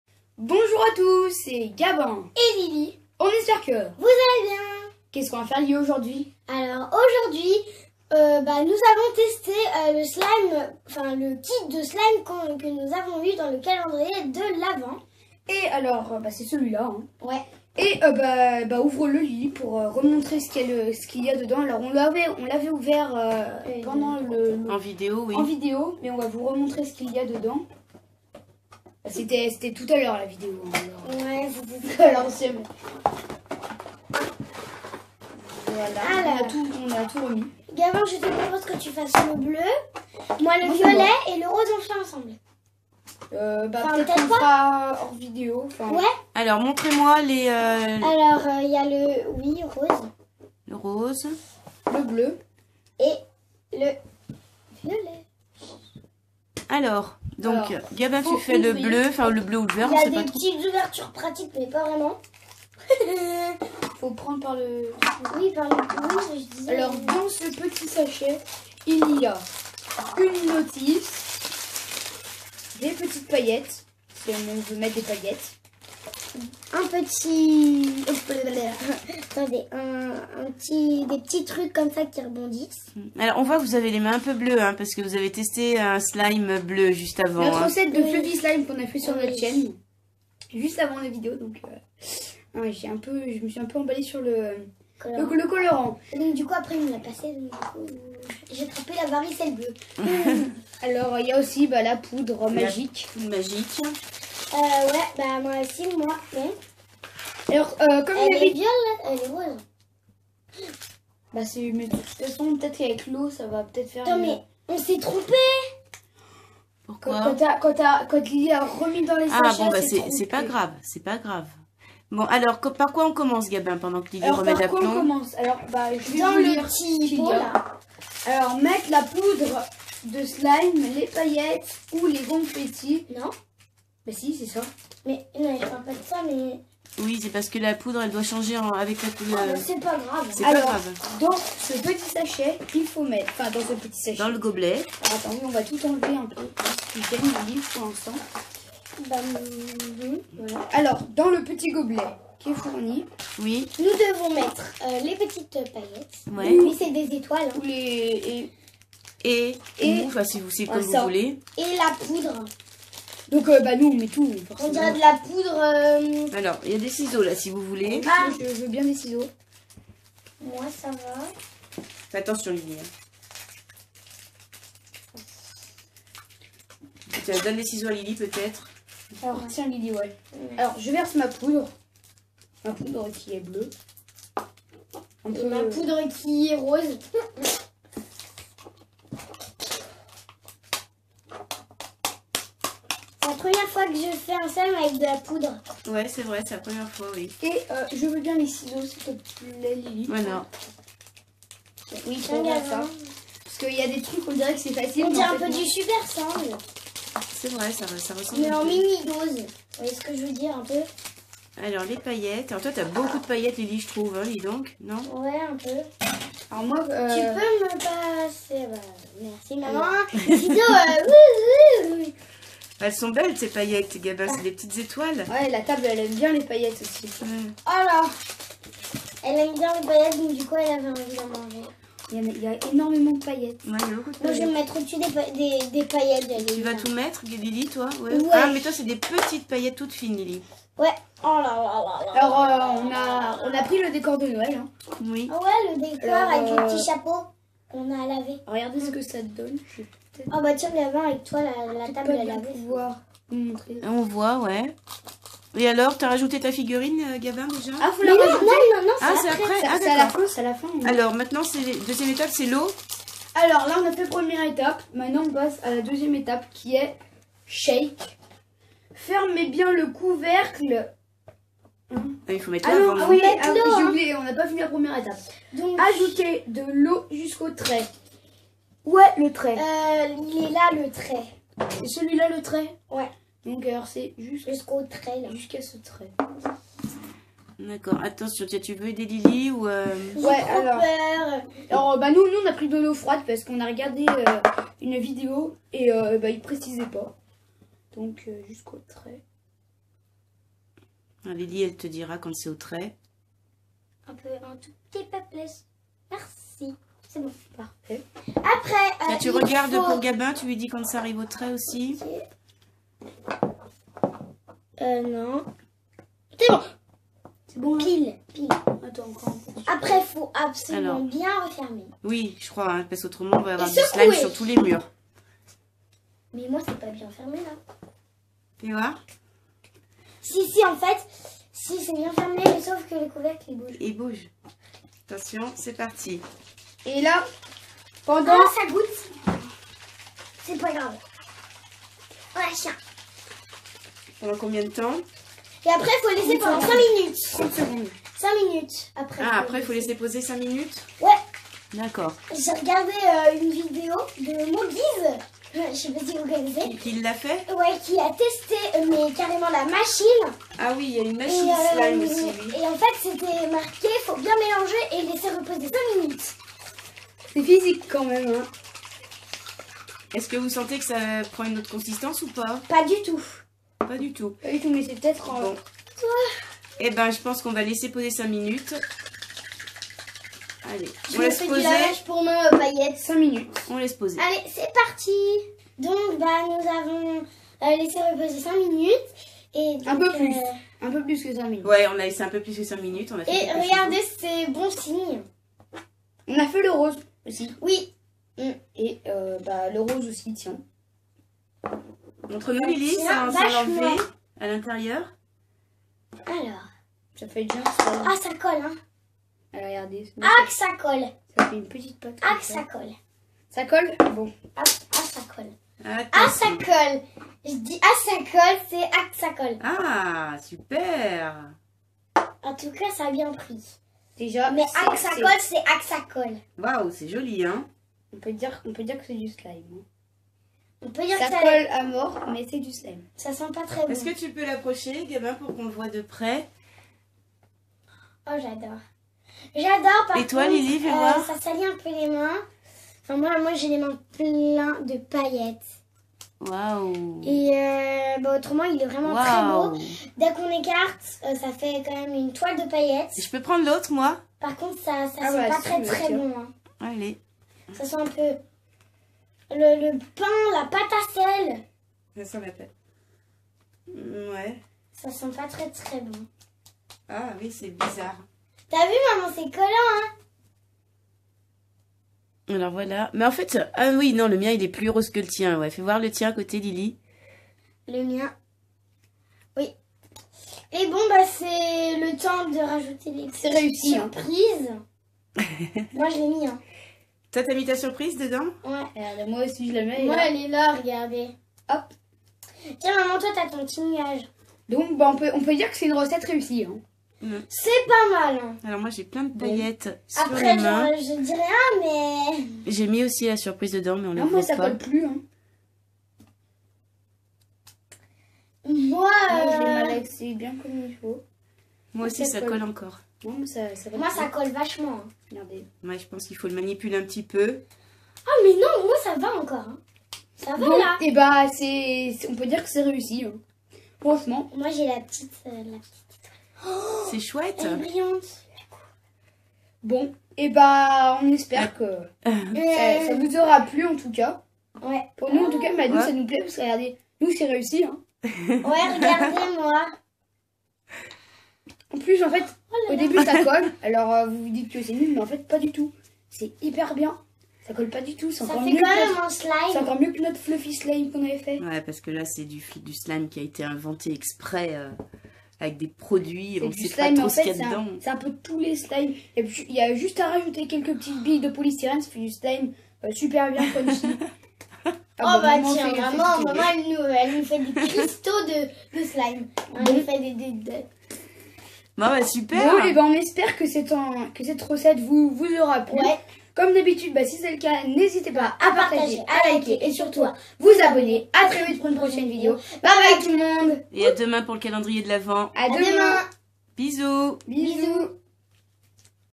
Test : Slime DIY Shakers
Aujourd'hui on teste le SLIME DIY SHAKERS, on kiffe de ouf!!